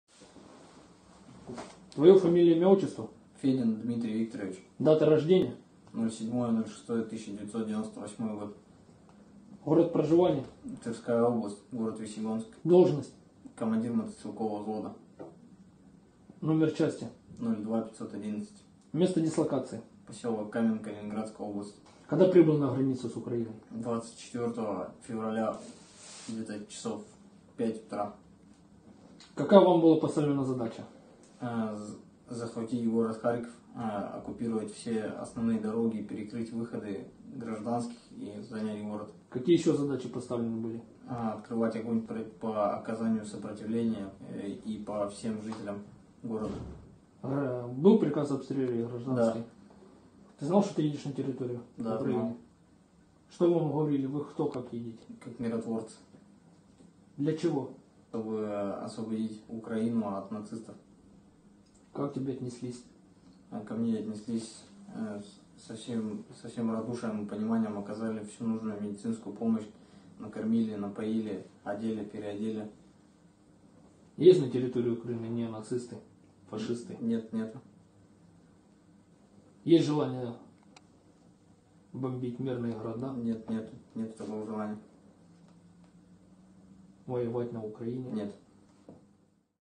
Russian soldier tells the truth about russia's war with Ukraine
He was taken prisoner and this is the first occupier of the russian federation who began to tell the truth about the orders and goals of the invasion of Ukraine.

Ukraine will win!